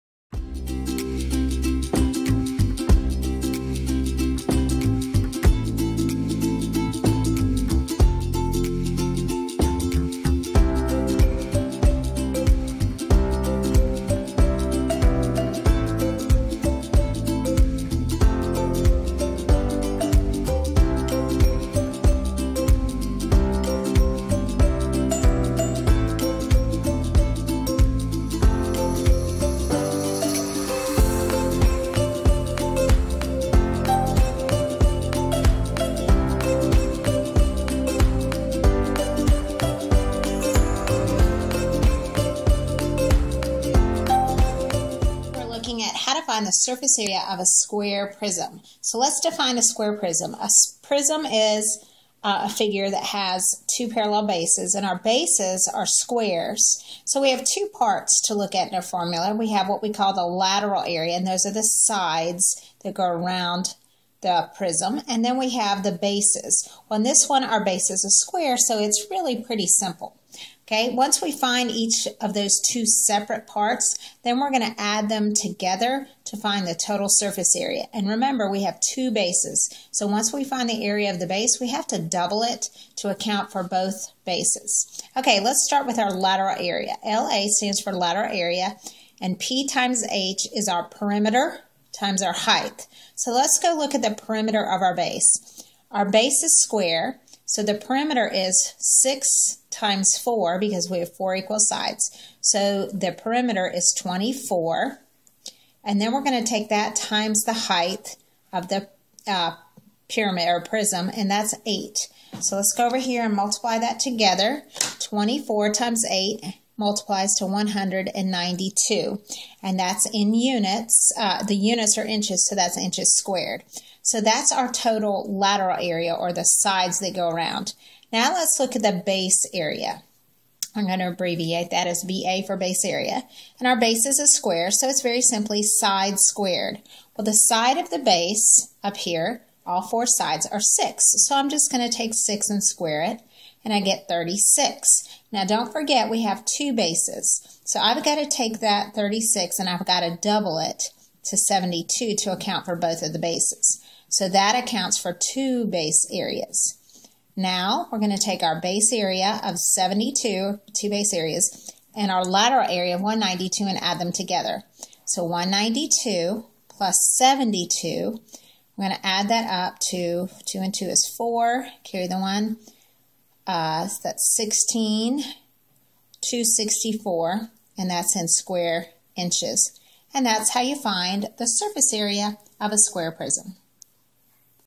How to find the SURFACE AREA OF A SQUARE PRISM
Learn how to find the surface area of a square prism.
The formula you can use is Lateral Area + 2(Base Area)
The lateral area equals perimeter times height
Base area equals length times width or side squared.